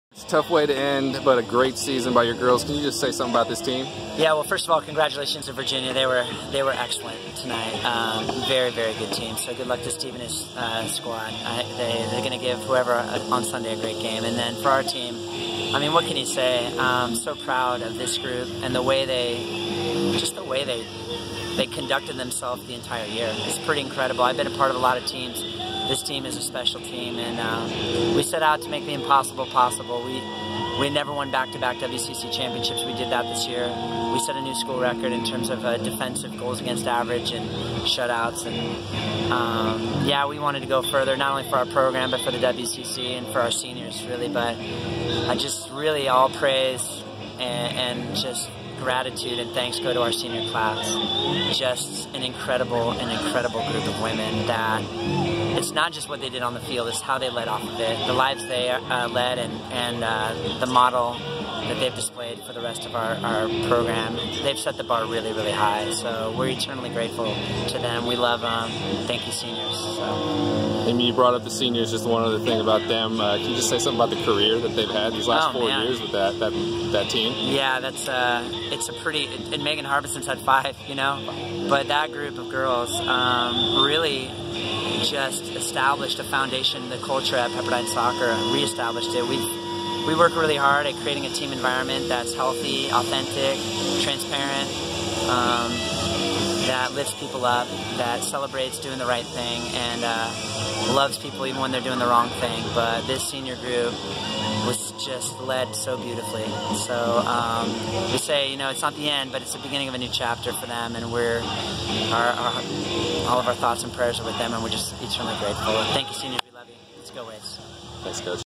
Women's Soccer - postgame interview with Head Coach Tim Ward
Women's Soccer - postgame interview with Head Coach Tim Ward following NCAA Tournament loss to Virginia.